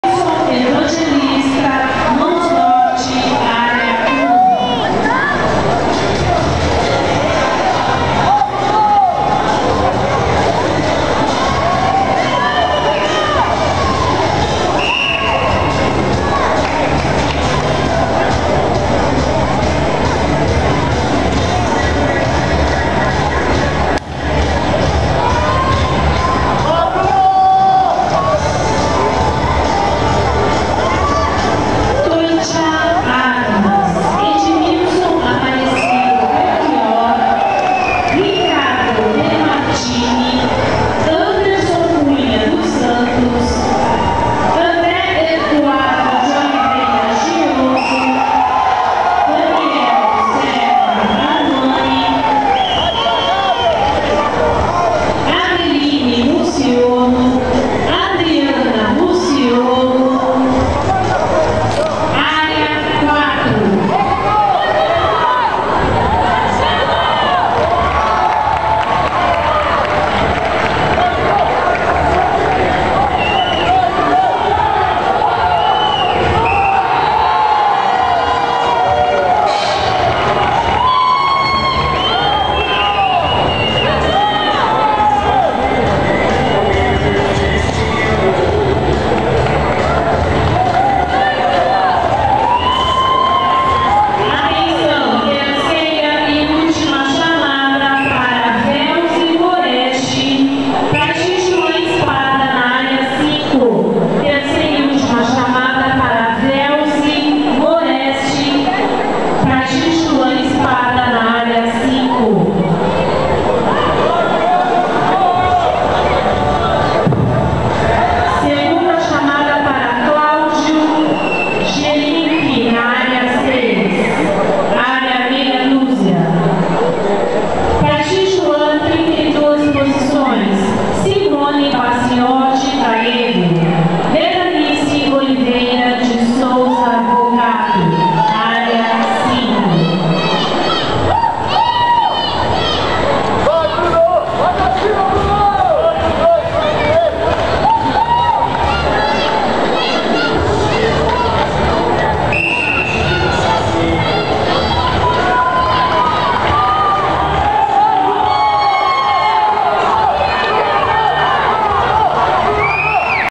espada tai ji a espada recordista invicta desde sua estreia em 1998#taijiespada#kungfu#artemarcial
Mestra Velzi Moreschi defendendo seu título na Arte do Tai Ji Espada Linhagem Mestre Liu Chih Ming
A beleza, a arte e elegância da arte marcial chinesa brilha em campeonatos oficiais no Brasil através da performance da Mestra Velzi Moreschi duas vezes octadecacampeã internacional 2014
Mestre Velzi Moreschi treina atletas medalhistas, ministra cursos de formação profissional, capacitação e especialização em Medicina Tradicional Chinesa - Qi Gong - Tai Chi Chuan-
contato